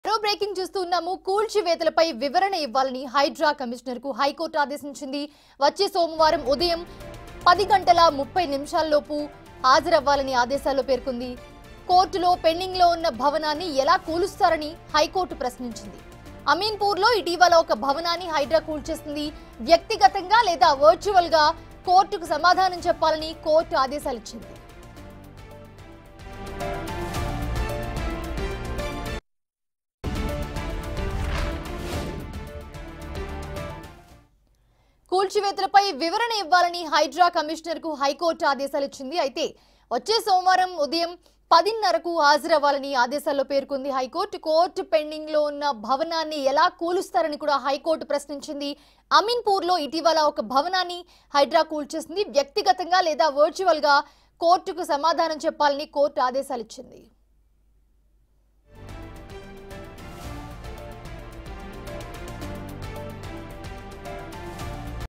రంగనాథ్ కు హైకోర్టు నోటీసులు | TS High Court Notices to Hydra Commissioner Ranganath | BIG TV
రంగనాథ్ కు హైకోర్టు నోటీసులు | TS High Court Notices to Hydra Commissioner Ranganath | BIG TV Hyderabad